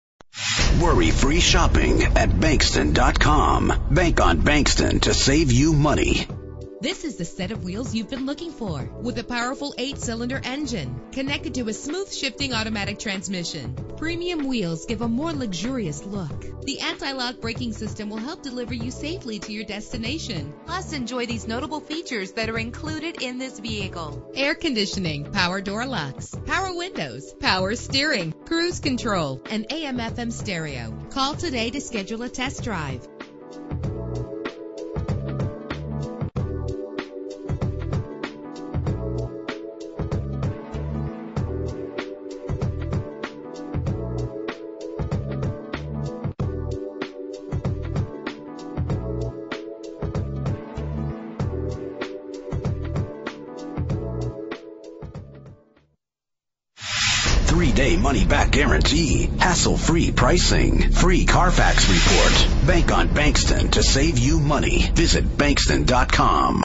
1996 Chevrolet Suburban - Dallas TX
Phone:
Year: 1996
Make: Chevrolet
Model: Suburban
Trim: 1500
Engine: 5.7 liter 8 cylinder 16 valve
Transmission: 4-Speed Automatic
Color: Brown
Mileage: 168387
Address:
4747 LBJ Freeway